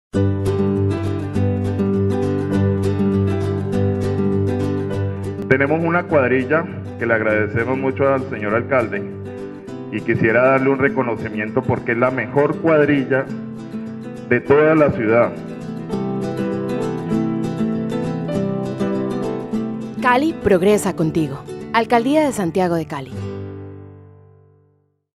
Solucionando las peticiones de la gente.
Habitante de la comuna 2 agradece al Alcalde, Maurice Armitage, por su gestión en el mejoramiento de las vías en la comuna 2.